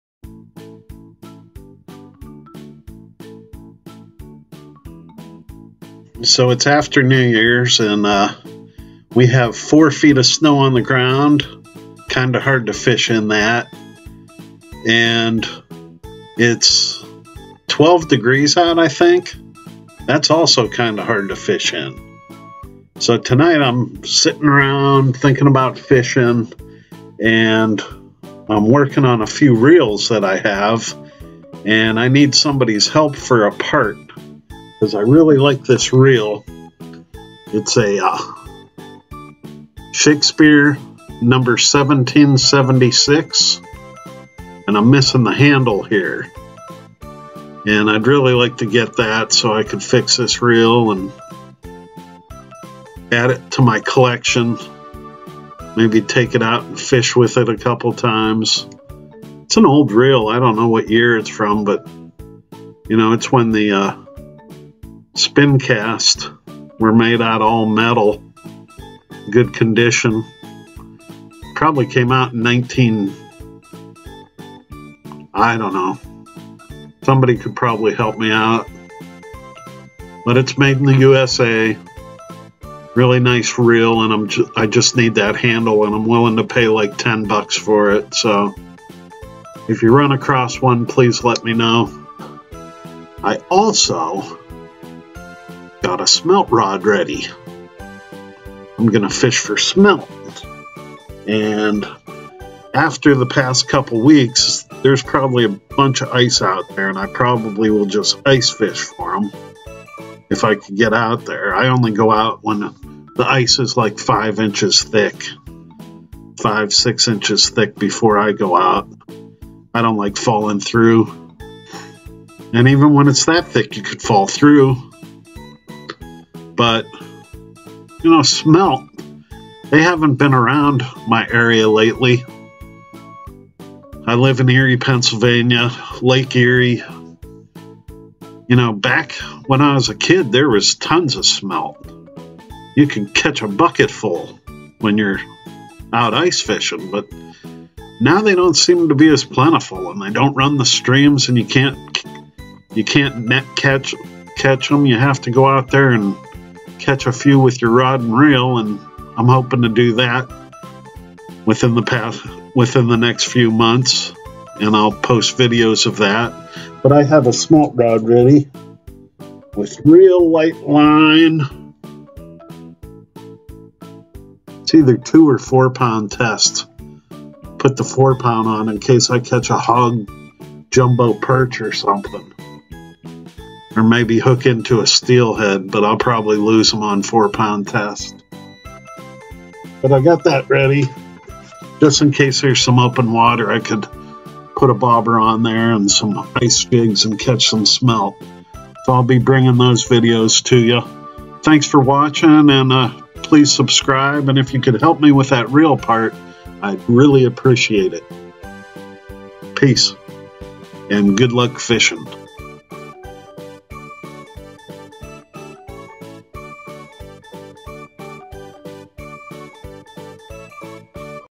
Dreaming of Smelt Fishing in the Dead of Winter
I want to get out there smelt fishing but the temperature is 12 degrees and the snow is 4 feet high.  So I got a fishing pole ready and worked on a few old reels.  Wanted:  A handle for a Shakespeare WonderCast 1776 spincast fishing reel like this reel here: